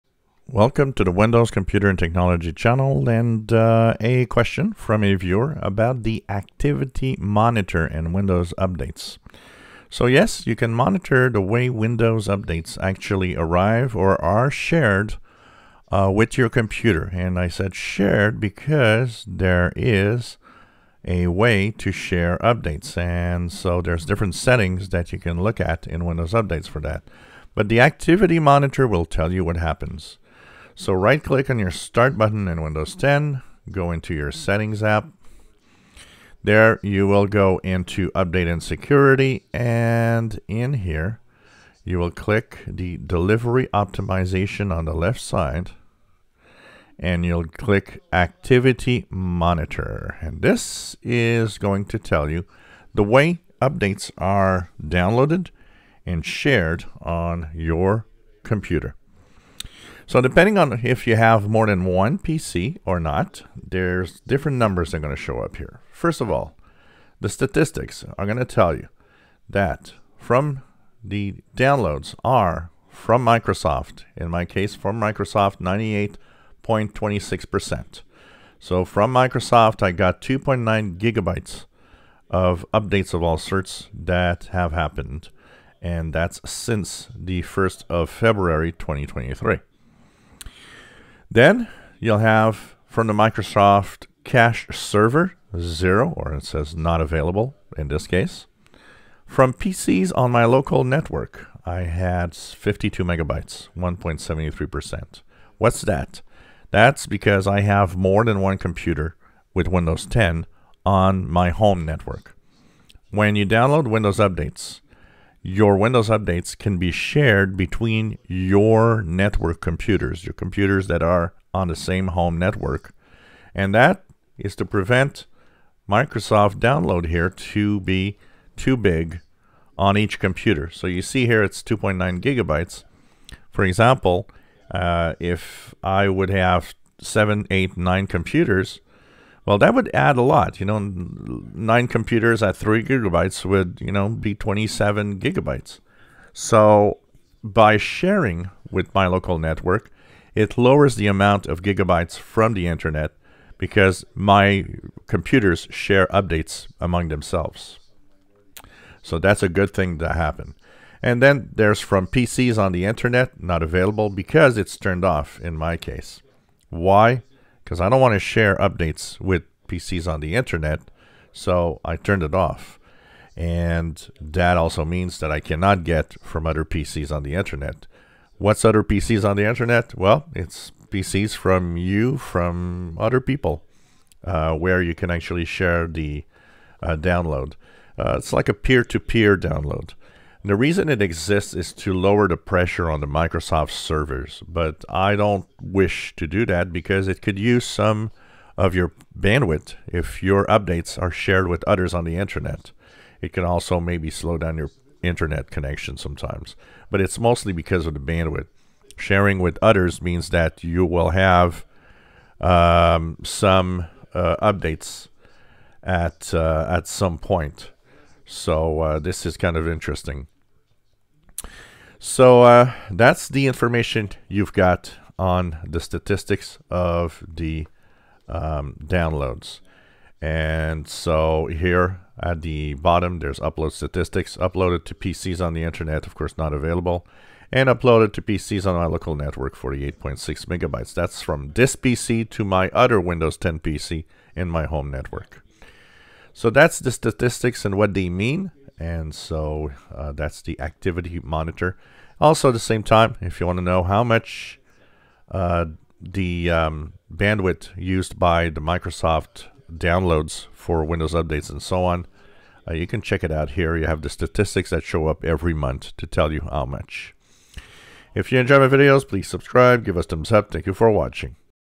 Windows 10 Windows update activity monitor is useful to check bandwidth use
Interesting information can be had from the Activity monitor in Windows updates